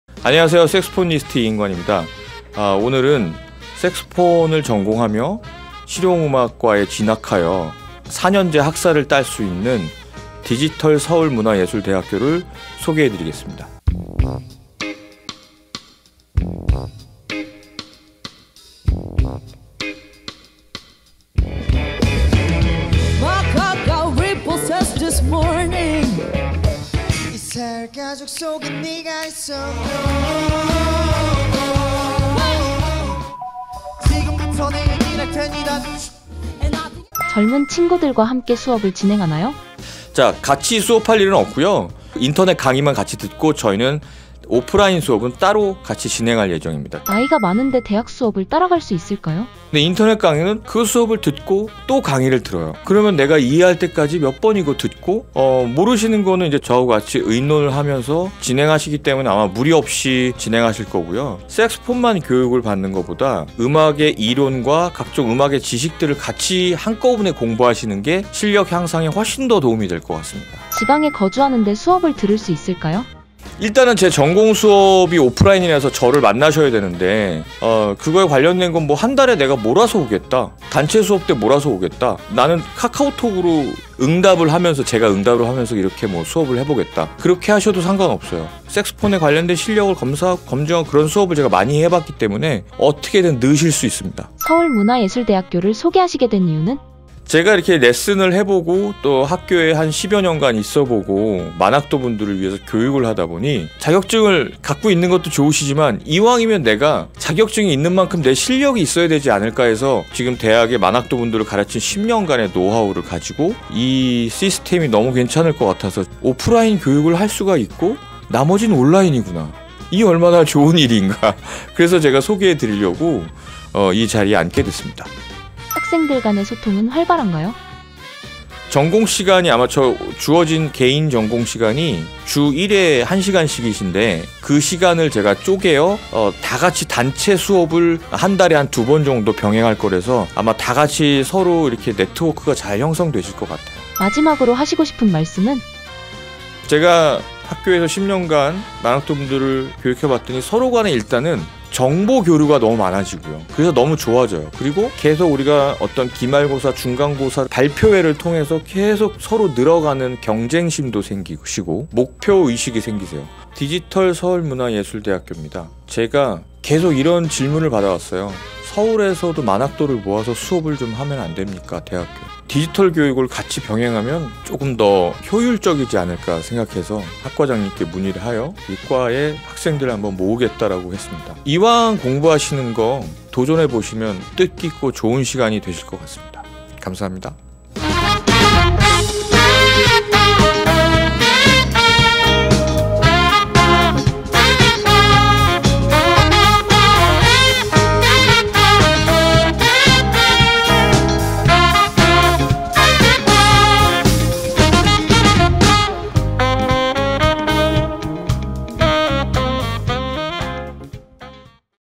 색소폰 자격증 따는 법?? - 색소폰 연주자 이인관 프로가 알려주는 색소폰 자격증 따는 법(서울문예대 실용음악학과)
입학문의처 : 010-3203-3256